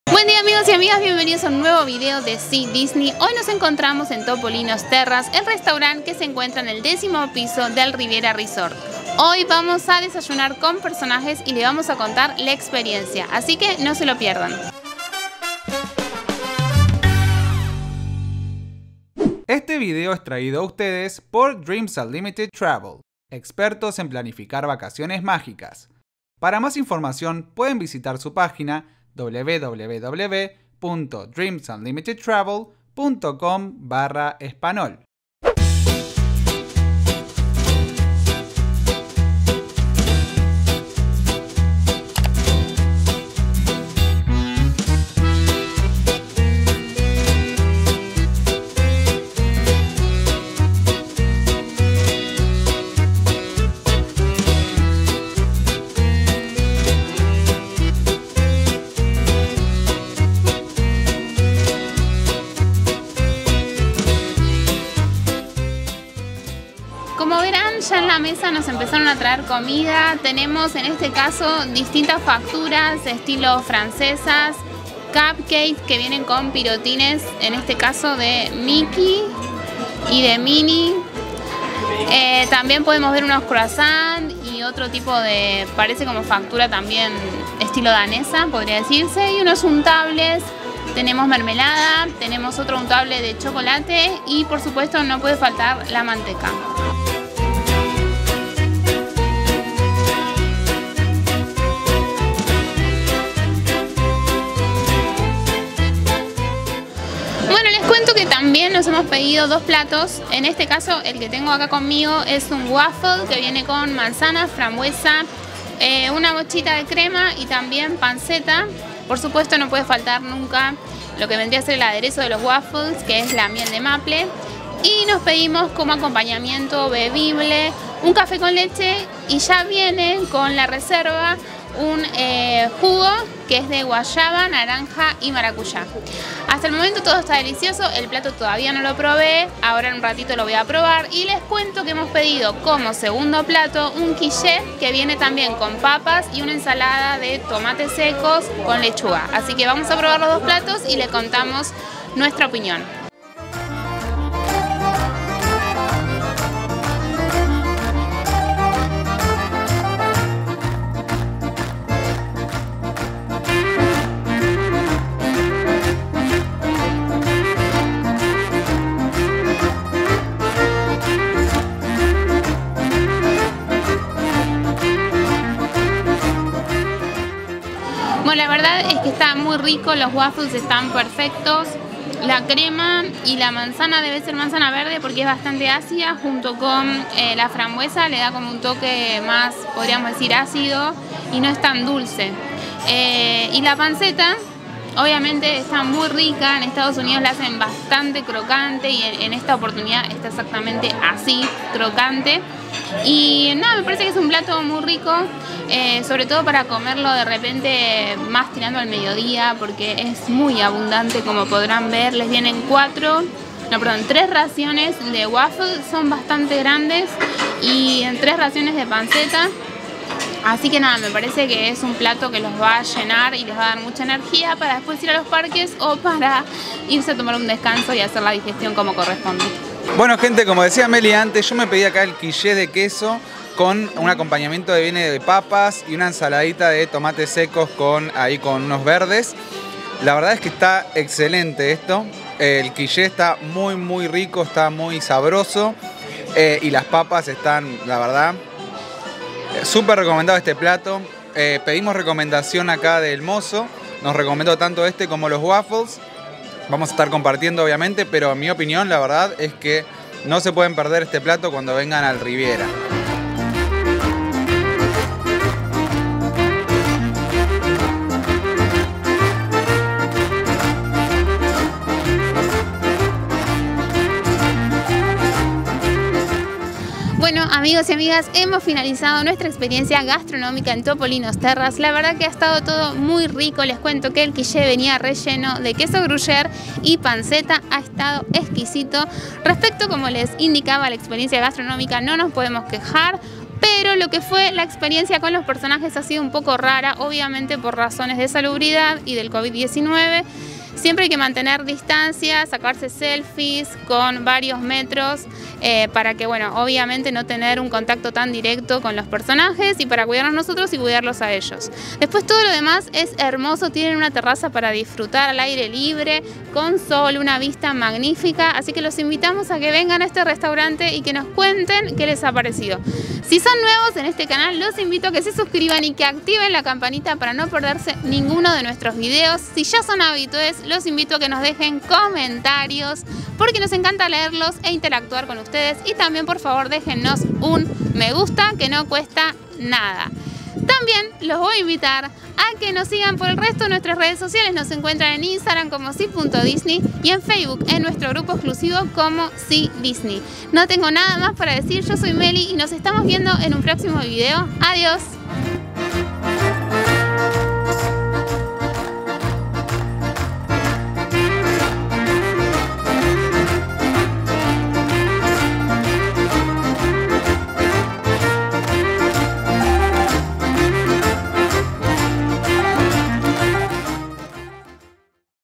Reseña de desayuno con personajes en Topolino's Terrace
Acompaña a Jimmy y a Meli a probar uno de los mejores restaurantes de Disney World y de los pocos que ofrece actualmente comidas con personajes: Topolino’s Terrace en el Disney’s Riviera Resort.

Para una cotización sin compromiso de unas vacaciones a Disney contacta uno de los agentes especializados de Dreams Unlimited Travel

SiDisney es parte del grupo The Dis.